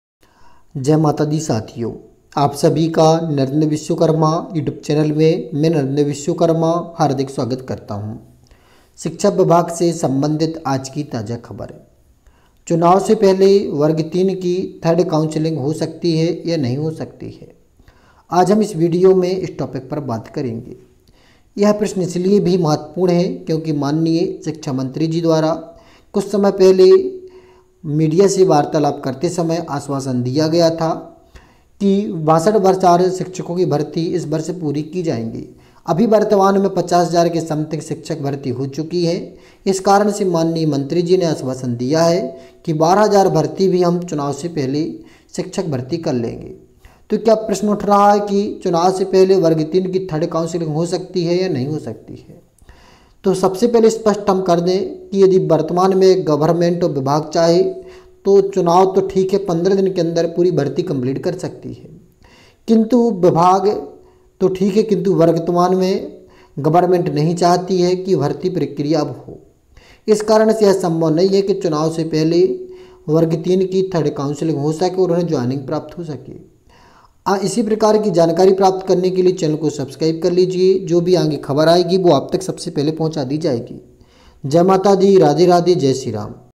chunav se pahle varg 03 ki third counseling ho sakti hai ya nahi 28/8/2023 ?
shiksha vibhag se sambandhit aaj ki taja khabar 28/08/2023

           🙏🙏 धन्यवाद साथियो 🙏🙏